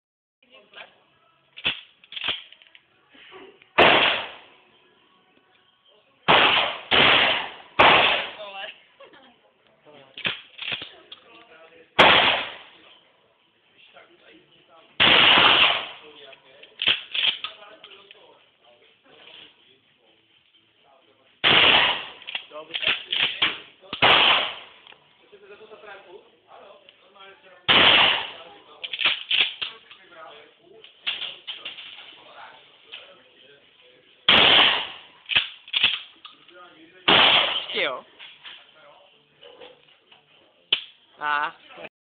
Pretty red hair girl firing a shotgun
This video was uploaded from an Android phone. By La Femme Nikita.